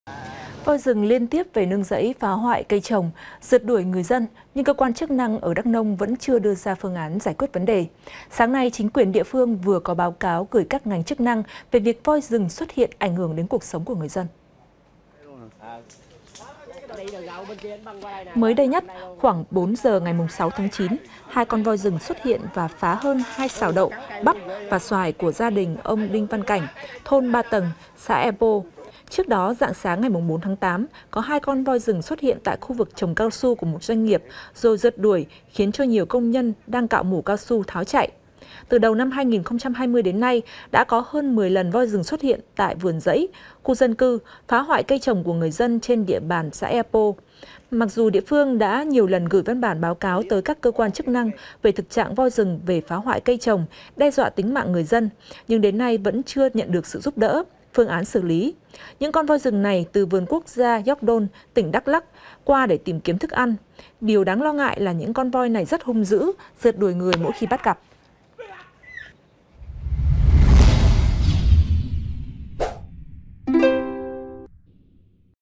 Voi rừng phá rẫy, rượt đuổi người dân ở Đắk Nông | VTC14
VTC14 | Voi rừng phá rẫy, rượt đuổi người dân ở Đắk Nông
Voi rừng liên tiếp về nương rẫy phá hoại cây trồng, rượt đuổi người dân nhưng cơ quan chức năng ở Đắk Nông vẫn chưa đưa ra phương án giải quyết vấn đề. Sáng nay chính quyền địa phương vừa có báo cáo gửi các ngành chức năng về việc voi rừng xuất hiện, ảnh hưởng đến cuộc sống người dân.